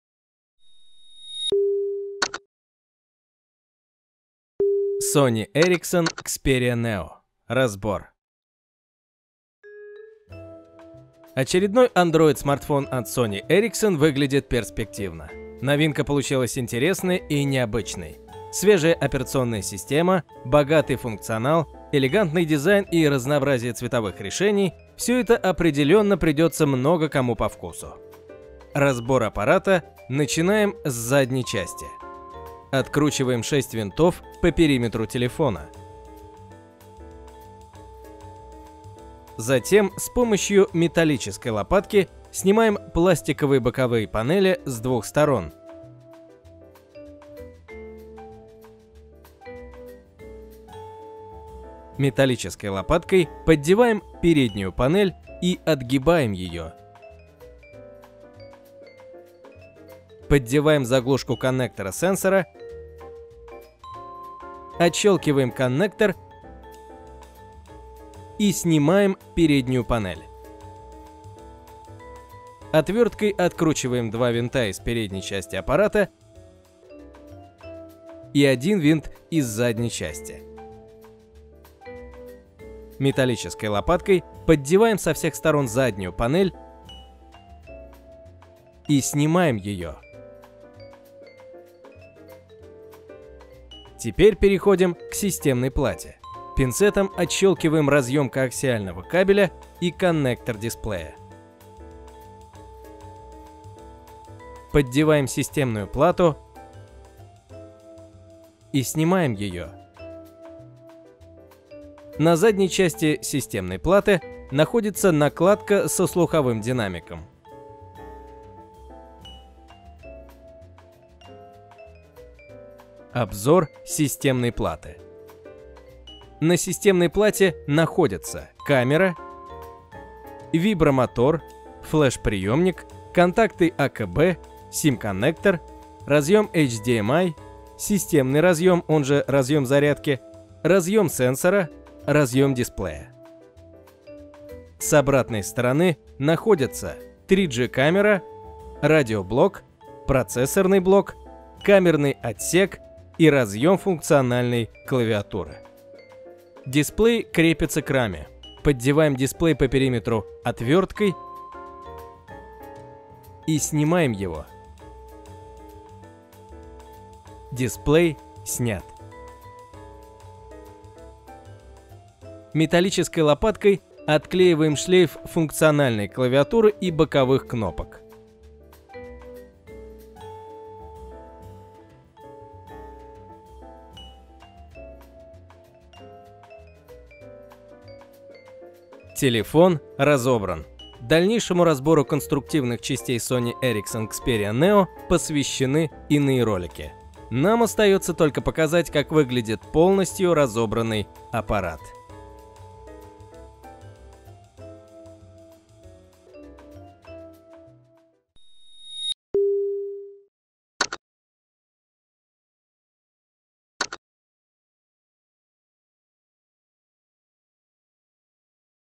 Sony Ericsson Xperia NEO - как разобрать телефон, из чего состоит
Подробное пошаговое описание разбора мобильного телефона Сони Эриксон Иксперия Нео: как разобрать Sony Ericsson Neo своими руками и произвести ремонт мобильного телефона самостоятельно.